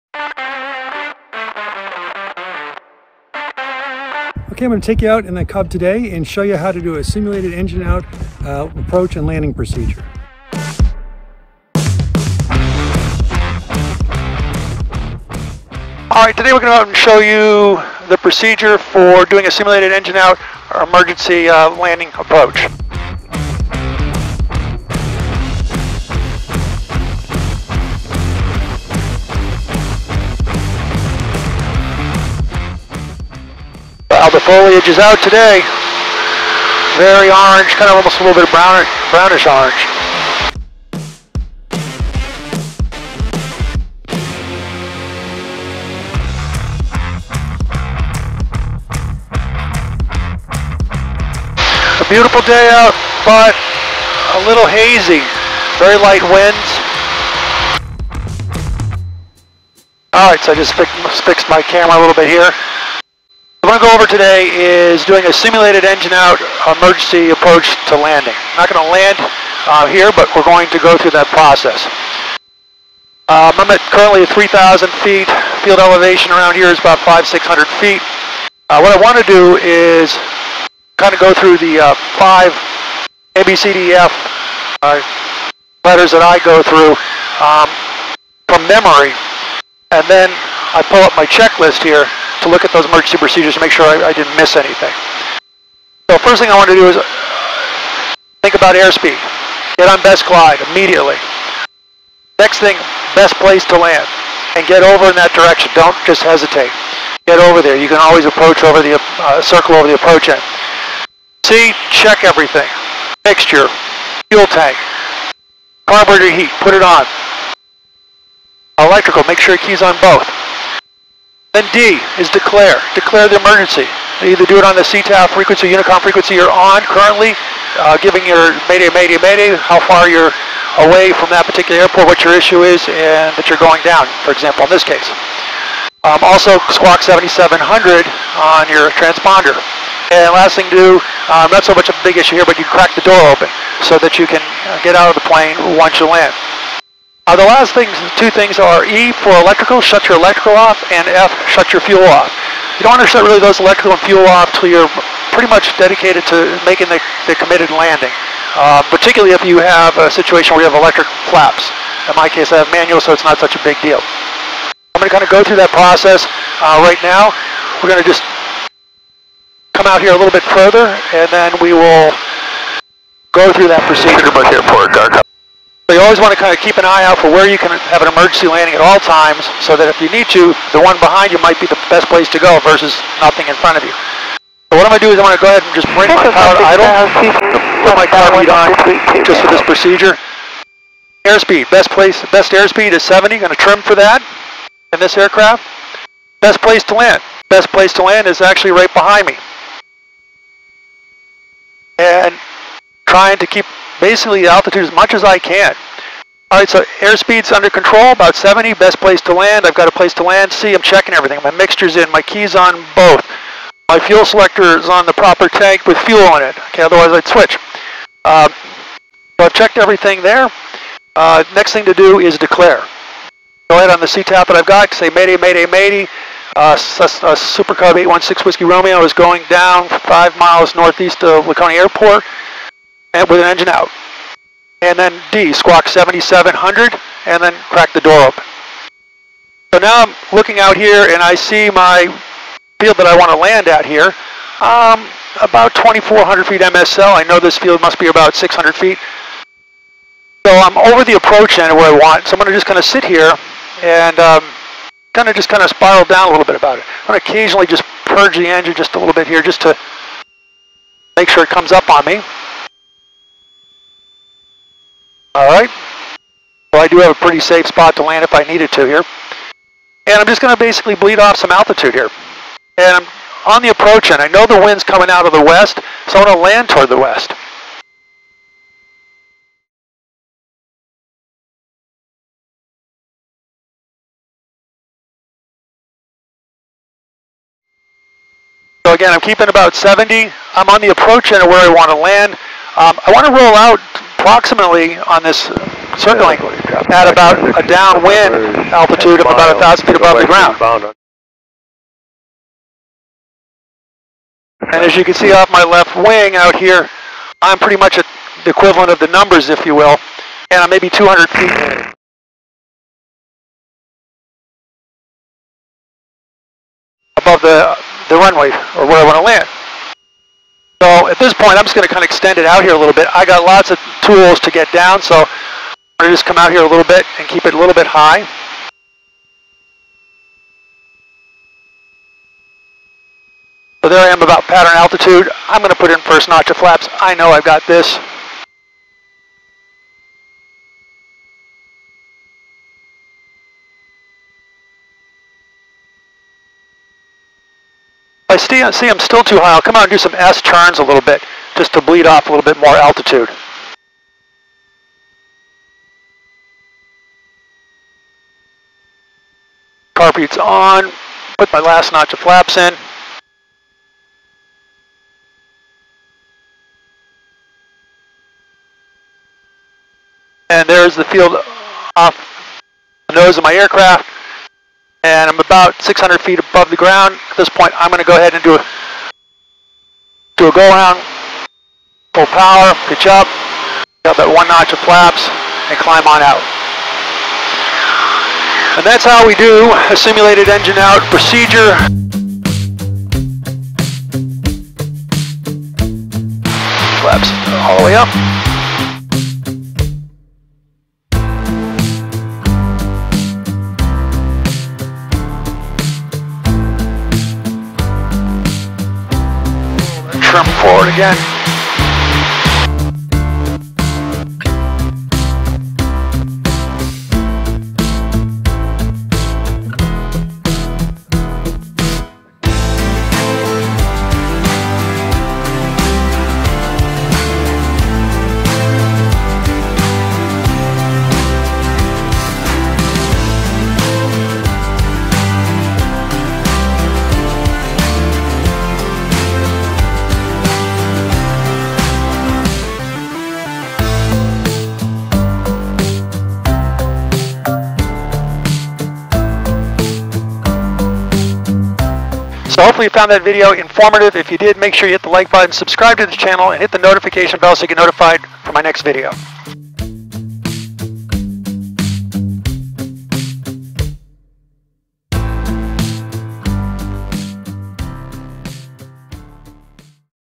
Emergency Engine Out Procedure in Super Cub
In this video I demonstrate an Emergency Engine Out Approach to landing procedure in a Piper Super Cub. As I mention in the video I utilize my (A,B,C,D,E,F) approach for memorizing the Emergency Engine Out procedure. After performing the emergency engine out procedure from memory, I also go back and check the Emergency Engine Out Checklist to confirm I did not missing anything.

Belos is my A,B,C,D,E,F Emergency Engine Out procedure that I have committed to memory:

AIRSPEED – Pitch for Glide Speed (and Trim)

BEST PLACE TO LAND – Find and start flying towards best place to land.

CHECK – Door to Floor Check (Key on Both, Primer, Fuel Selector, Aux Fuel Pump, Mixture, Carb Heat)

DECLARE – Call on CTAF or 121.5MHZ Frequency and Declare emergency (May Day…), and Squawk 7700

When committed to the landing (e.g. on Final)

ELECTRICAL OFF – After putting in your last notch of flaps for landing, Shut off Electrical (Master SW & Ignition SW)

FUEL OFF – Fuel Selector Valve off, Mixture Idle Cut-Off

Land Aircraft at Short Field Landing Approach Speed